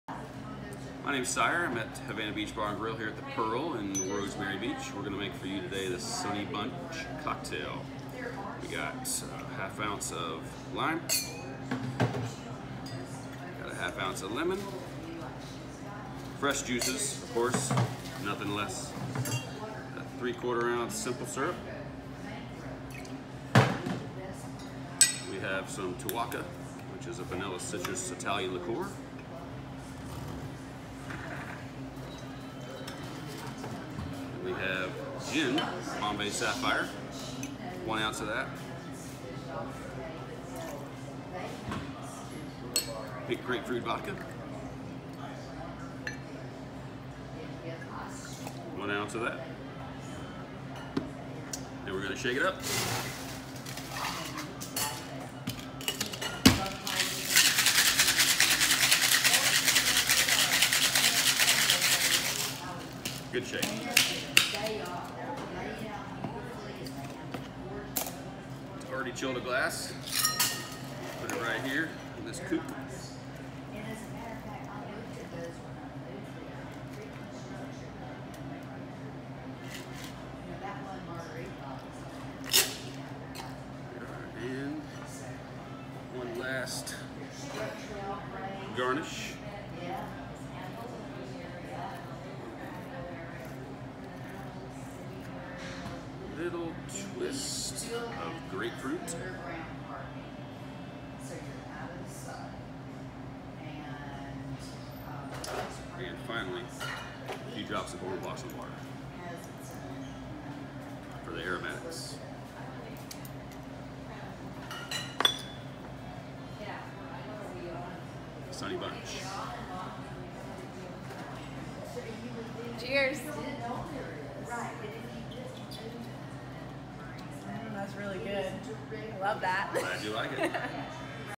Make This Beach Cocktail - 30A The Pearl Hotel Rosemary Beach
Watch as Sire Negri from Havana Beach Bar & Grill  (inside The Pearl Hotel in Rosemary Beach, Florida) makes The Sunny Bunch cocktail, a refreshing summer citrus delight!

🏖️☀️FUN 30A LINKS👙🌴